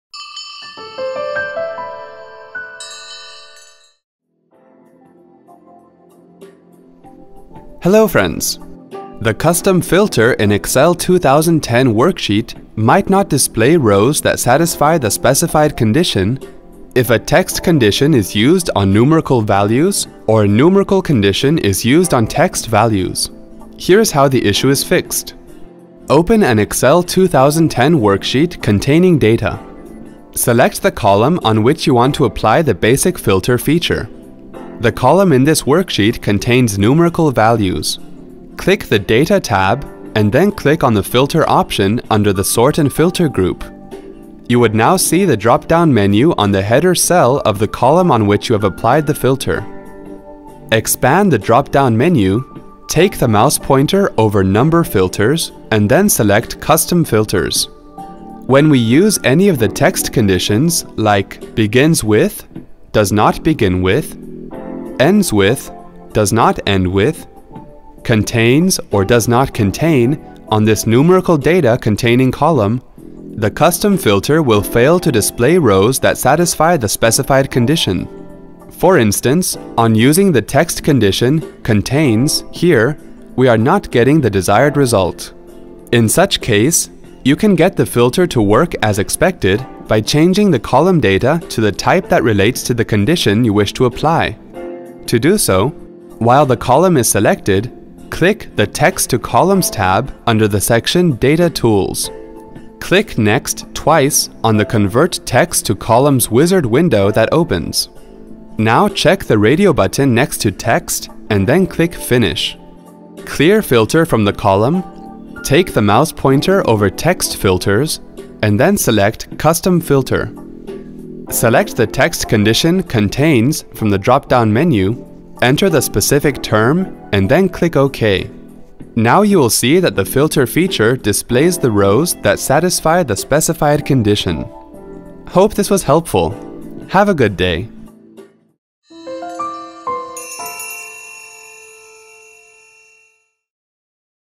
How to filter rows in Microsoft® Excel 2010
Fix the issue when a 'Custom' filter fails to display rows that satisfy the specified condition while working on a Microsoft® Excel 2010 worksheet in a Windows® 7 PC by following the steps shown in this video.